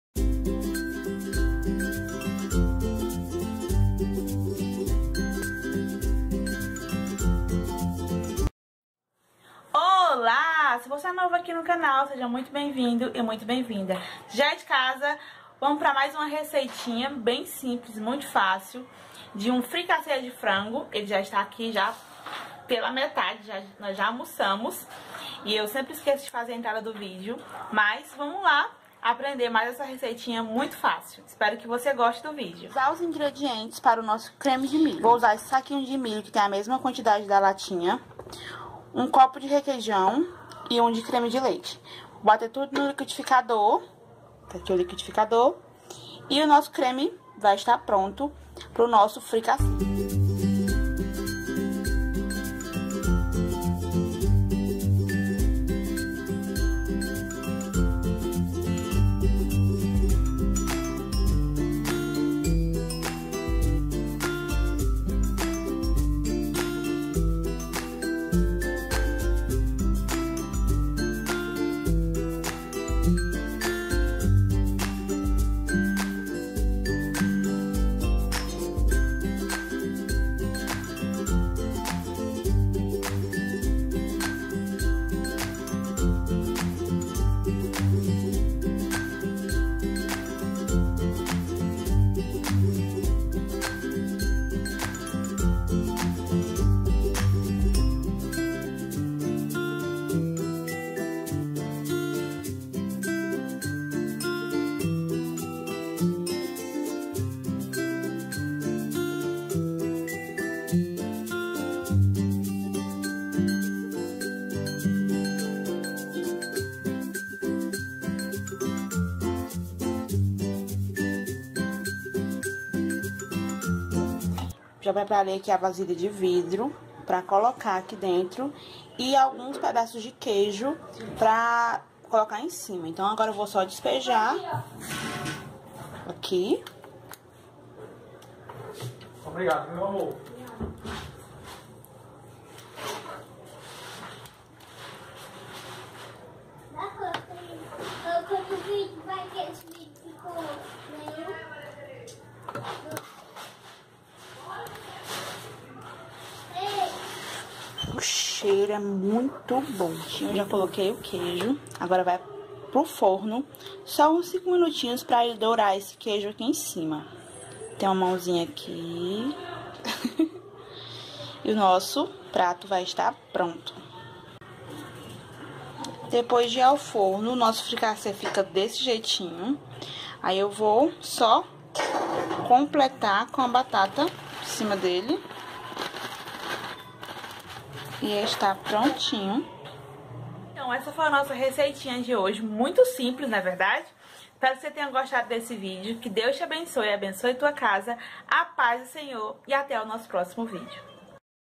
FRICASSE DE FRANGO FÁCIL E DELICIOSO
Olá amigaaa domingo fiz um FRICASSE DE FRANGO , É TÃO SIMPLES E GOSTOSO que vim compartilhar com você:)
TODO MUNDO AMOUUUU , Cuidaaa e já faz amanhã pro almoço da familia .

Gostou do video ?